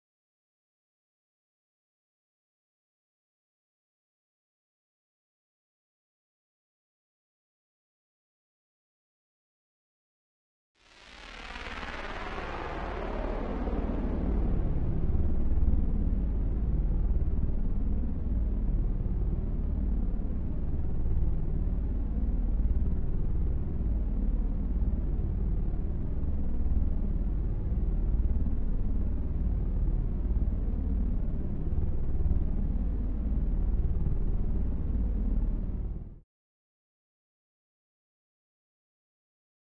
Asteroid 2012 DA14 February 15, 2013 Animation
Animation of a half football field sized Asteroid 2012 DA14 as it hopefully passes by Earth on Friday, February 15th, 2013 at about 14:24 EST